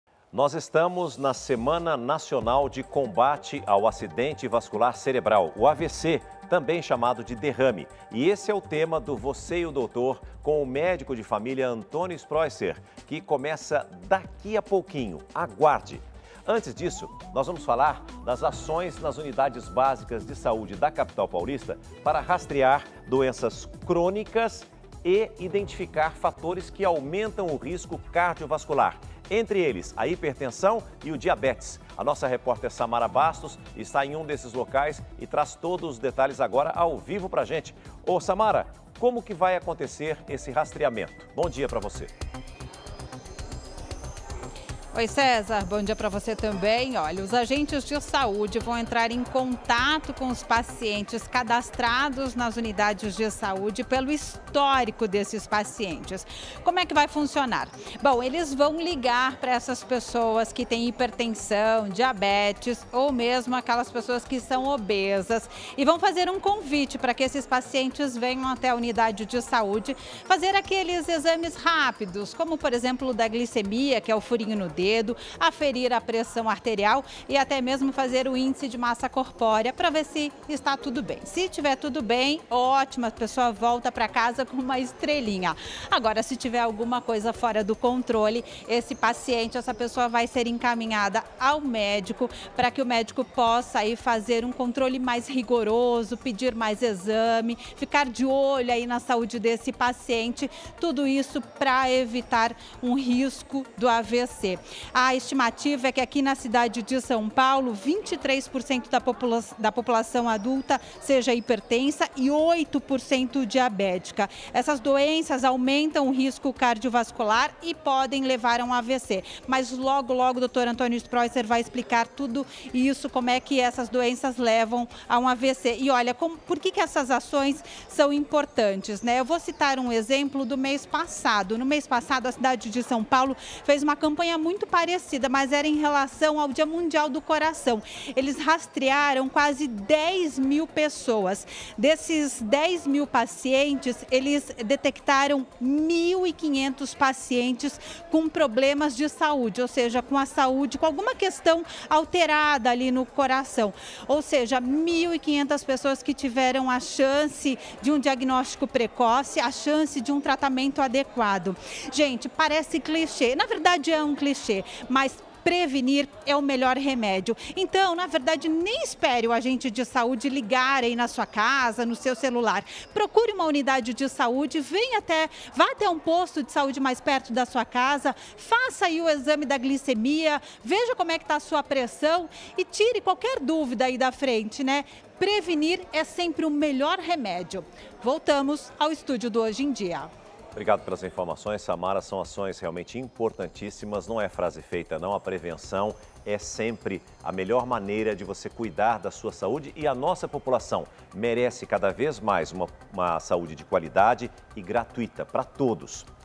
Ações em São Paulo rastreiam fatores de risco para o AVC
Nós estamos na semana nacional de combate ao Acidente Vascular Cerebral (AVC) também chamado de derrame. Ações nas Unidades Básicas de Saúde da capital paulista para rastrear doenças crônicas e identificar fatores que aumentam o risco cardiovascular. Entre eles, a hipertensão e o diabetes.